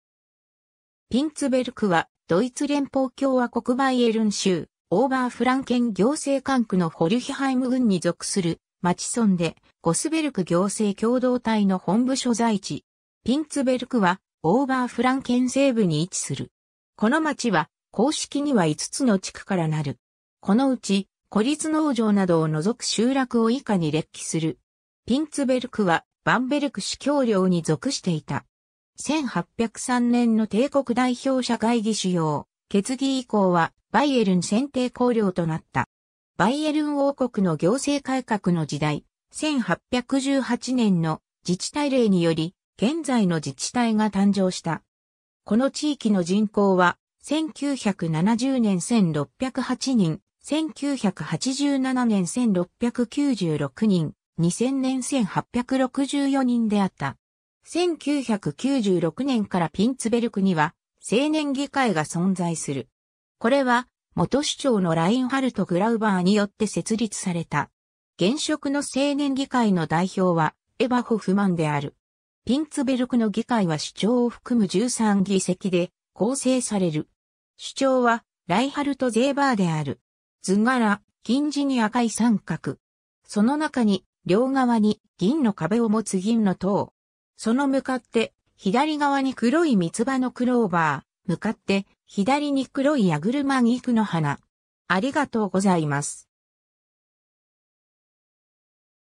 ピンツベルク
ピンツベルク (ドイツ語: Pinzberg) は、ドイツ連邦共和国バイエルン州オーバーフランケン行政管区のフォルヒハイム郡に属する町村（以下、本項では便宜上「町」と記述する）で、ゴスベルク行政共同体の本部所在地。
 ピンツベルクはオーバーフランケン西部に位置する。
 ピンツベルクは、バンベルク司教領に属していた。
1803年の帝国代表者会議主要決議以降は、バイエルン選帝侯領となった。
バイエルン王国の行政改革の時代、1818年の自治体令により、現在の自治体が誕生した。
 この地域の人口は、1970年 1,608人、1987年 1,696人、2000年 1,864人であった。
 1996年からピンツベルクには、青年議会が存在する。
これは、元首長のラインハルト・グラウバーによって設立された。
現職の青年議会の代表はエヴァ・ホフマンである。
 ピンツベルクの議会は首長を含む13議席で構成される。
 首長は、ライハルト・ゼーバー（CSU/Bürgerblock）である。
 図柄: 金地に赤い三角。
その中に、両側に銀の壁を持つ銀の塔。
その向かって左側に黒い三つ葉のクローバー、向かって左に黒いヤグルマギクの花